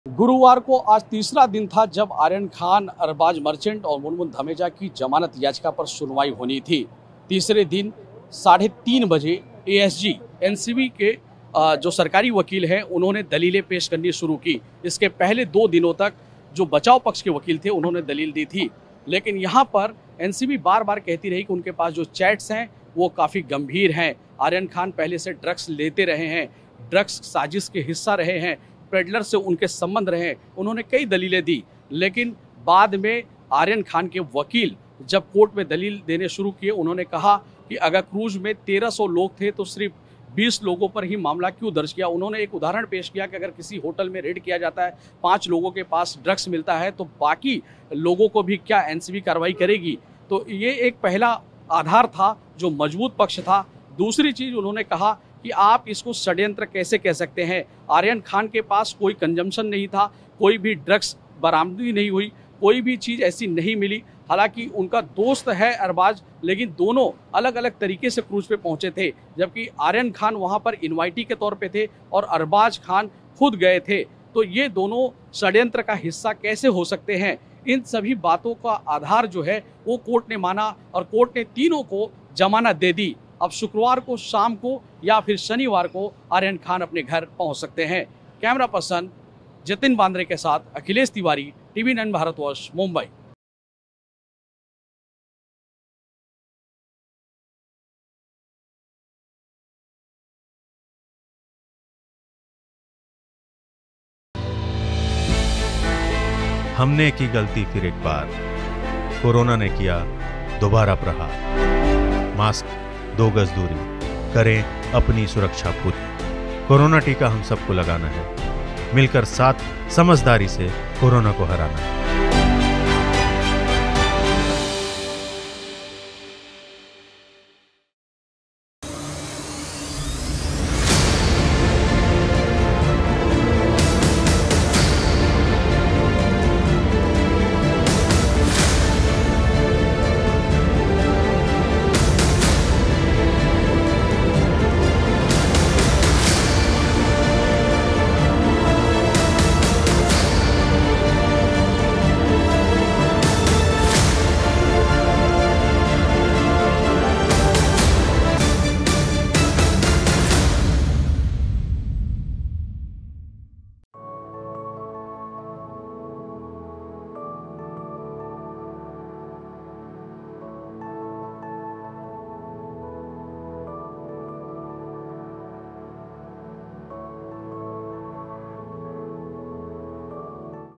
सुनवाई के दौरान Aryan Khan के वकील ने रखी कई अहम बातें, जानिए वकील ने क्या दी दलील ?
आर्यन की वकील मुकुल रोहतगी ने एनसीबी द्वारा आर्यन खान की गिरफ्तारी पर सवाल उठाए थे. उन्होंने कहा था- आर्यन खान की गिरफ्तारी का कोई आधार नहीं है. आर्यन के पास से कोई बरामदगी नहीं हुई थी.

Credit: National | Ashutosh Singh/Producer | Sumit/Editor